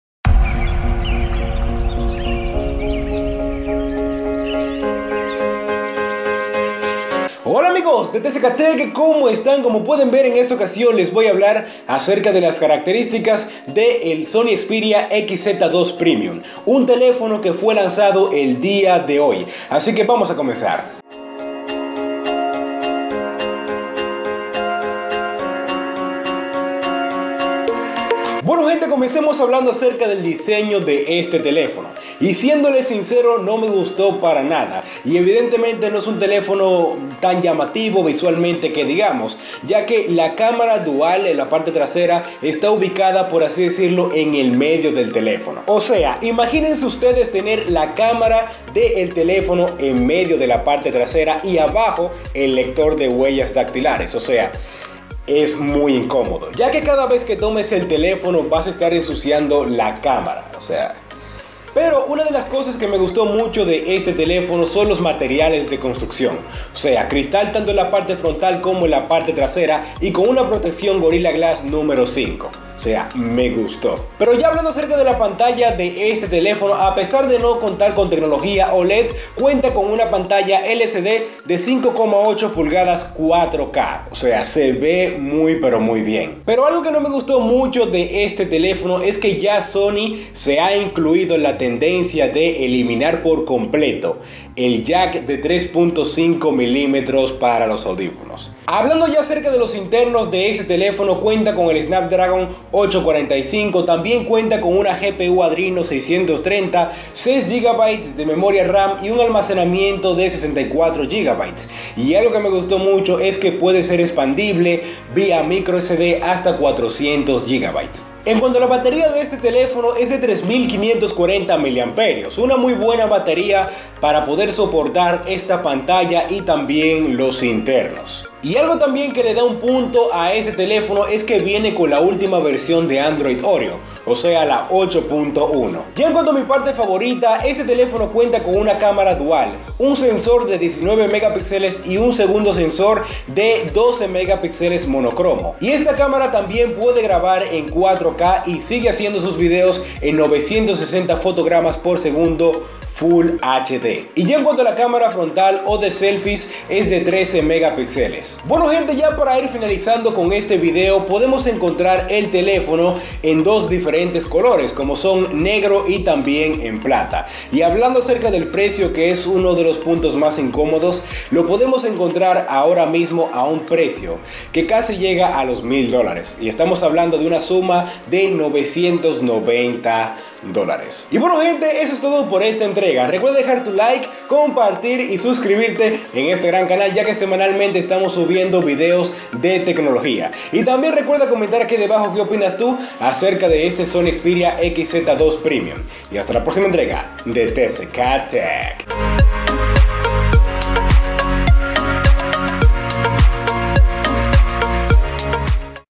Sony Xperia XZ2 Premium con Doble Camara!!! (Caracteristicas)
Sony Xperia XZ2 Premium un smartphone que trae muy buenas caracteristicas!!!